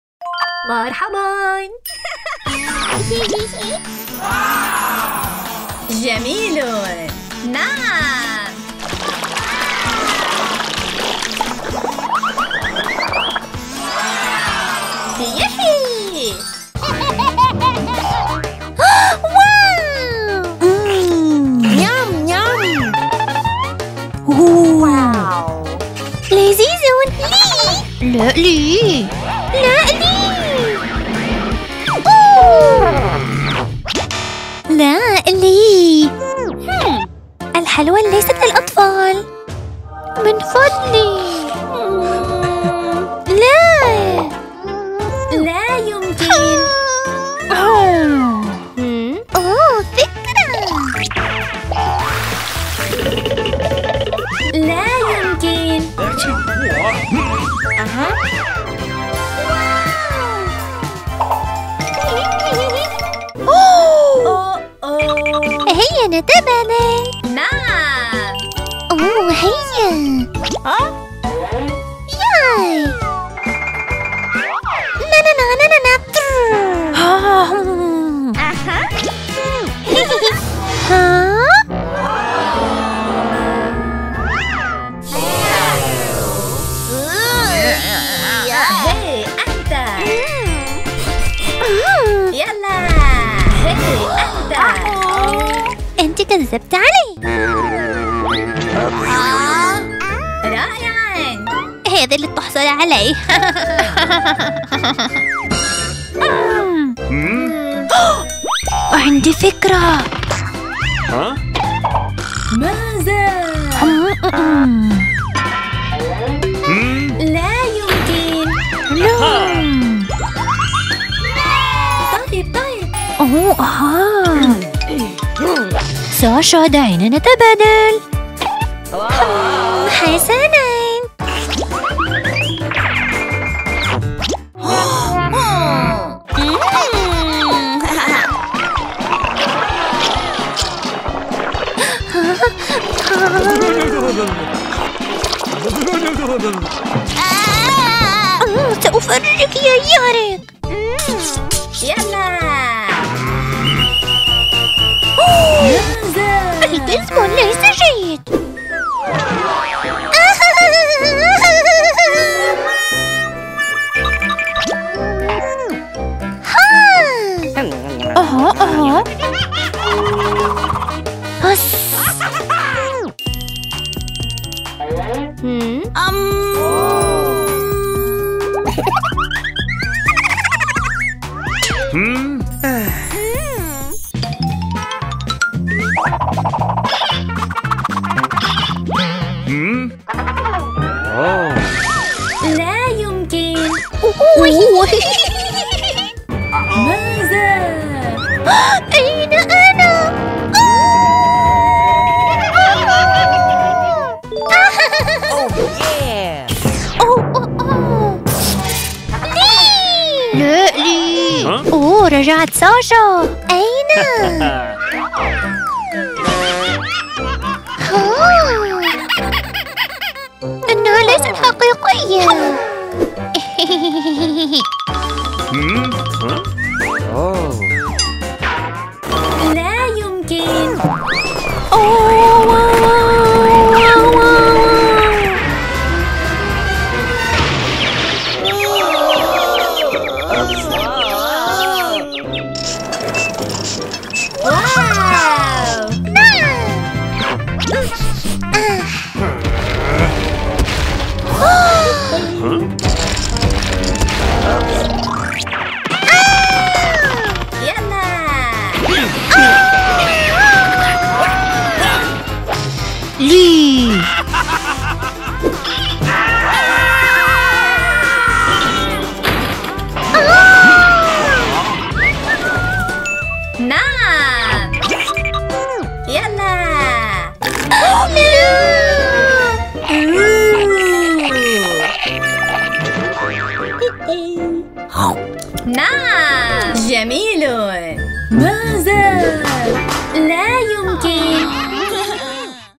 ساشا وأنيتا في تحدٍ مضحك للأطفال
ساشا تطبخ. أنيتا وياريك يساعدان ساشا